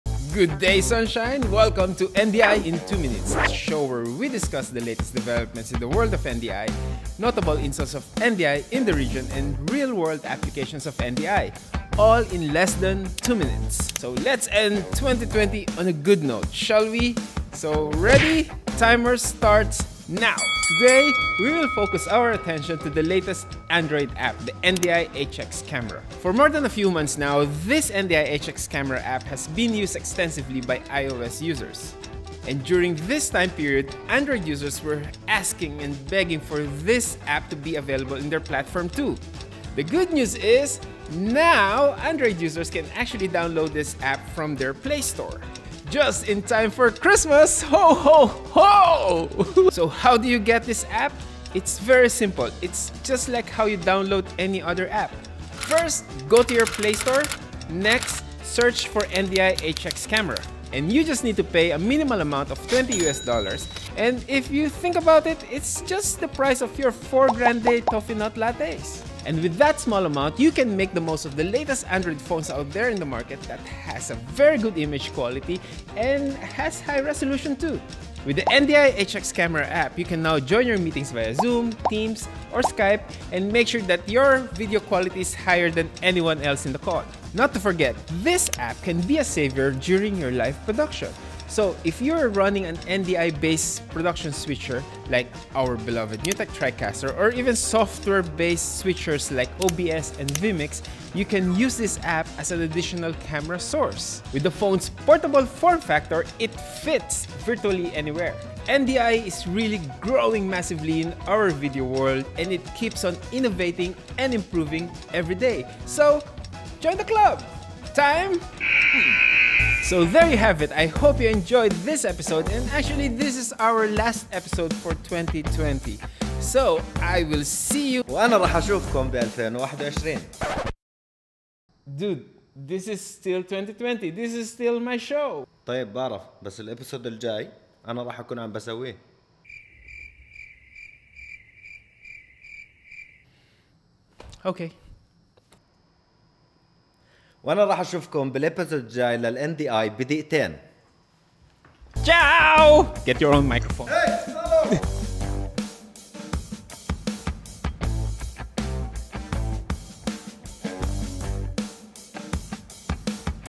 NDI in Two Minutes: NDI HX App for Android Devices!
Welcome to NDI in Two Minutes, the show about NDI for those that have busy schedules, short attention spans, or both.

In this video, we talk about the newly released NDI HX App for Android Devices! Previously available only to IOS users, the NDI HX App turns your mobile phone into another NDI Source that you can use and incorporate into your live production.

Watch the whole video to know more, including different applications and why this is another important step in NDI World Domination!